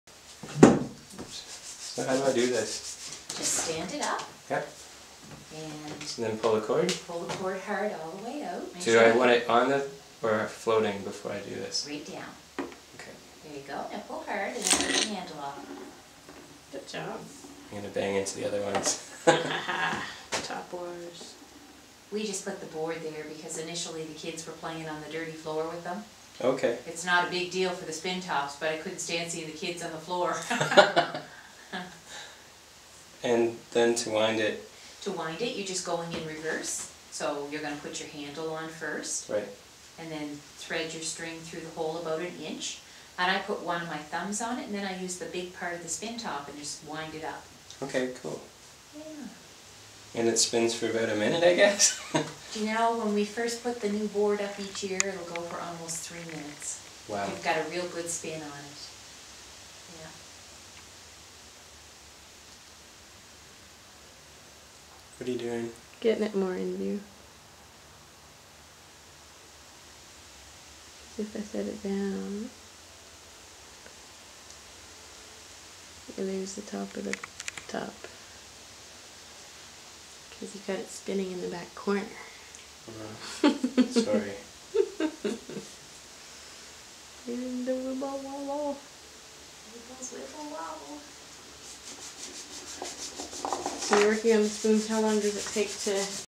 Sherbrooke Village, NS, Spinning Tops (1 of 2)
Sherbrooke Village is a living history museum in Nova Scotia.  They have a real blacksmith, an old fashioned printing press, a working water-powered sawmill, a potter, and a variety of other things, where all the people dress as people would over 100 years ago.  for more information.

This clip shows hand made spinning tops.